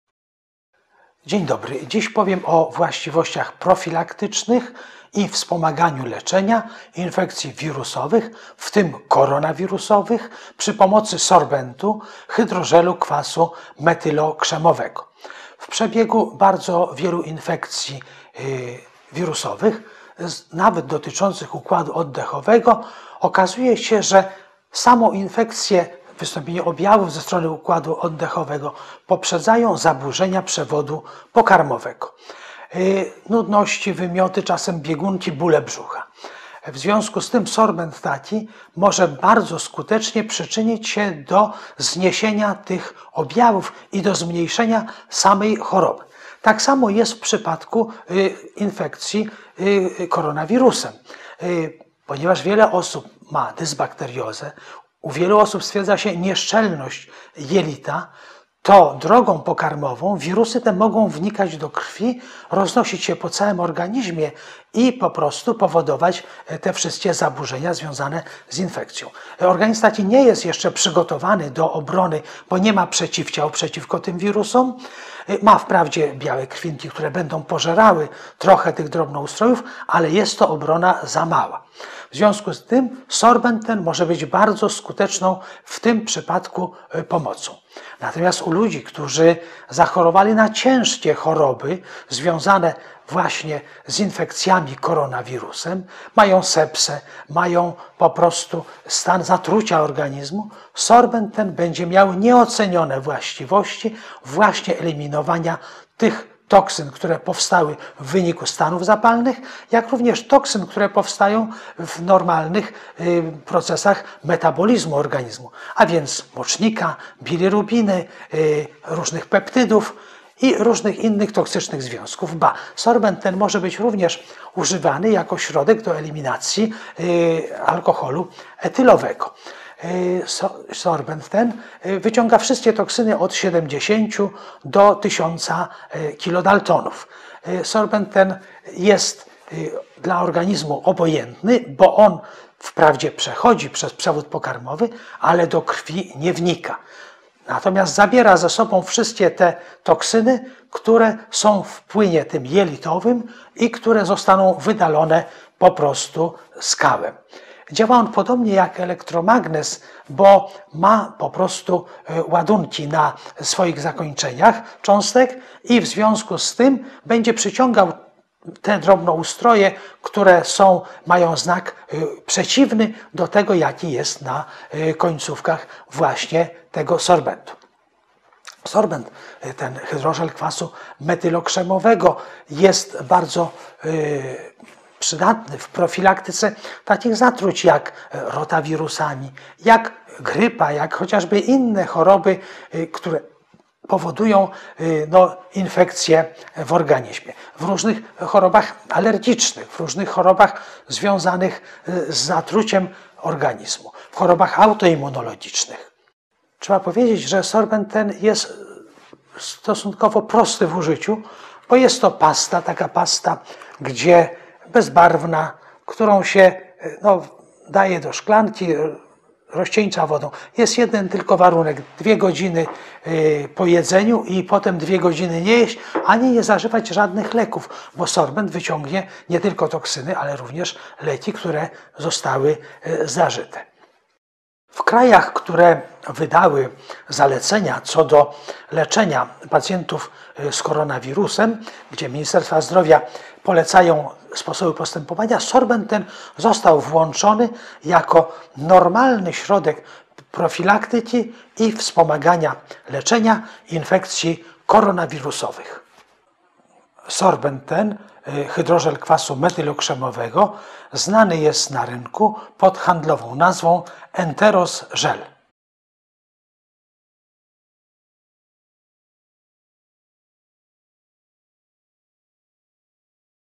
Dr L.M. Krześniak o profilaktycznych właściwościach i wspomaganiu leczenia chorób wirusowych
Dr L.M. Krześniak opowiada w felietonie o wspomaganiu leczenia infekcji wirusowych sorbentami krzemowymi - hydrożelami kwasu metylo-krzemowego (np. Enterosgel).
Enterosgel® to doustny jelitowy adsorbent (enterosorbent). Działa na bazie minerału organicznego ze zdolnością adsorpcyjną w odniesieniu do niektórych toksycznych i szkodliwych substancji w jelicie (takich jak endotoksyny, enterotoksyny): wiąże je fizycznie na swojej powierzchni i obniża tym samym częstość oddawania stolca oraz skraca utrzymywanie się biegunki. Enterosgel® jest wydalany z organizmu ze stolcem i w ten sposób usuwa adsorbowane toksyny i substancje szkodliwe, co może przynosić korzystne efekty w leczeniu wskazanych stanów ostrej biegunki i przewlekłej biegunki w zespole drażliwego jelita.